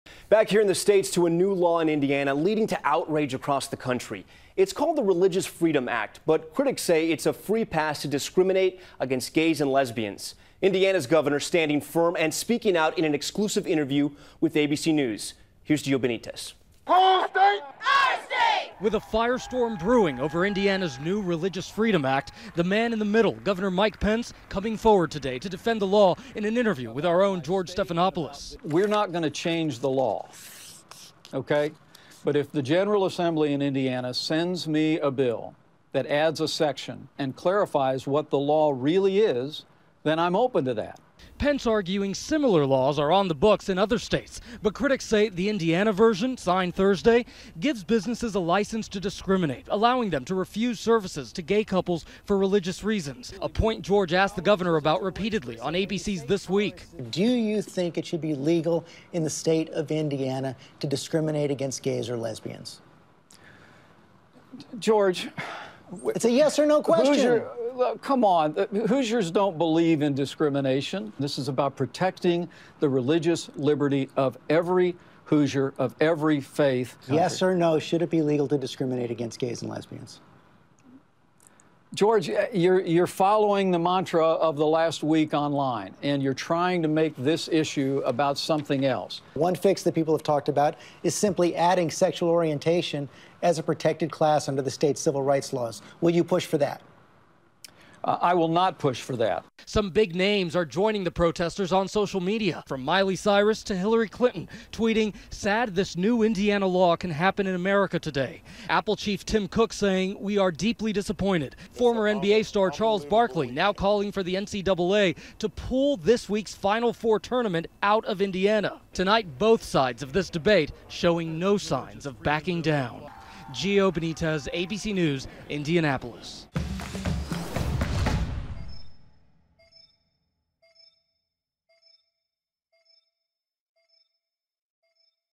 Mike Pence Defends Controversial 'Religious Freedom Act'
The Indiana governor vows not to change the law despite backlash.